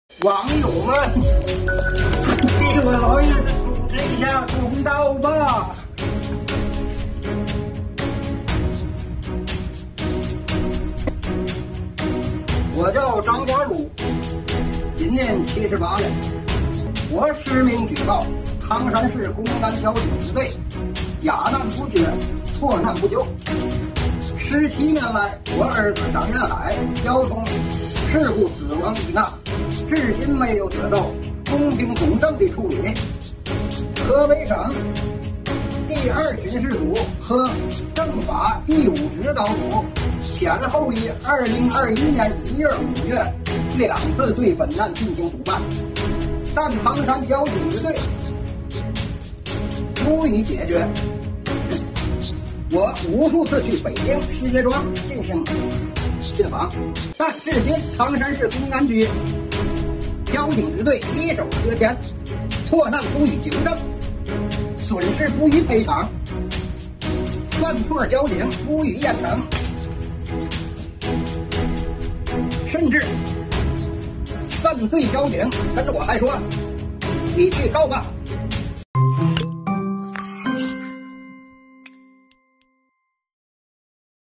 唐山78岁老人实名举报，儿子被撞死信访17年：相关部门不作为
视频日期；2022年6月13日 中国#2022#涉黑#河北#唐山 6月13日，河北唐山，一位78岁的老人张家儒，实名举报唐山市交警支队不作为，17年前老人儿子被车撞死，老人称因交警有证不取造成案件错判，河北省督导组对本案督导两次，唐山公安局一直不不予处理不给赔偿，老人信访17年，但交警支队错案不予纠正，损失不予赔偿，犯错交警不予严惩。- 今日头条唐山市夏季社会治安整治“雷霆风暴”专项行动举报方式 - 今日头条